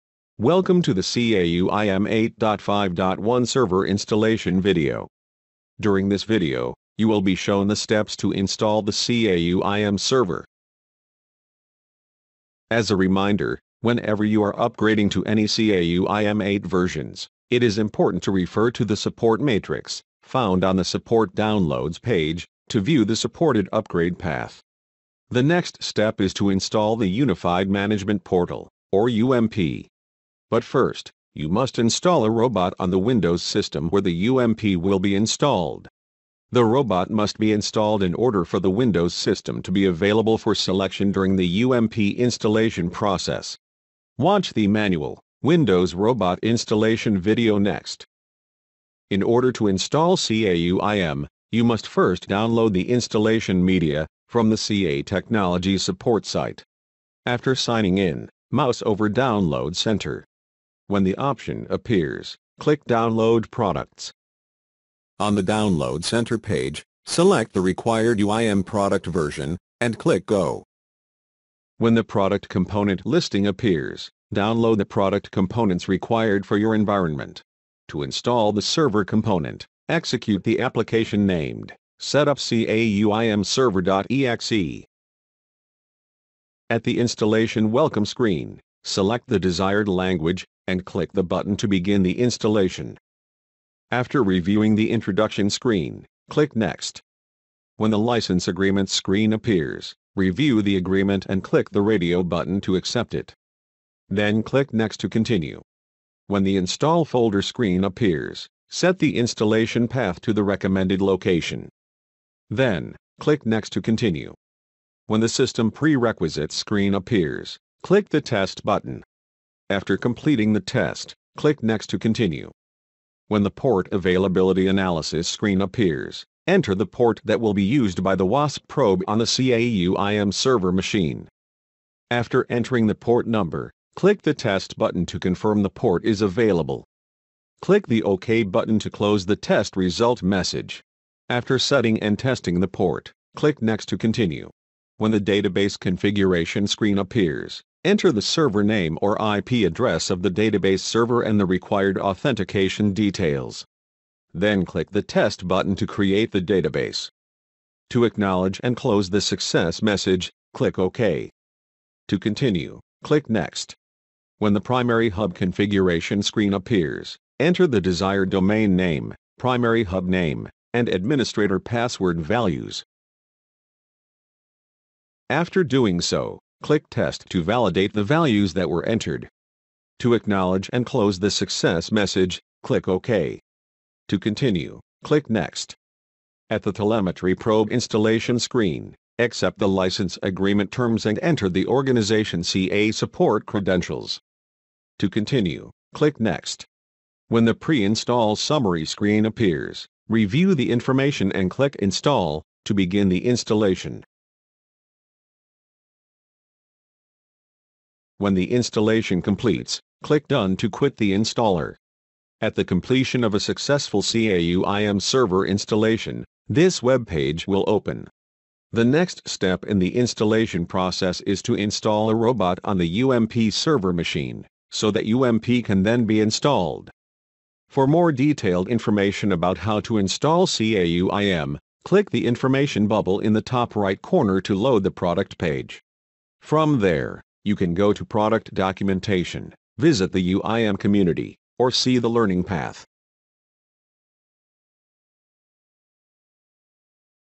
Install CA UIM Server 8.5.1
This video demonstrates how to install CA UIM Server 8.5.1, and should be the FIRST step in the installation process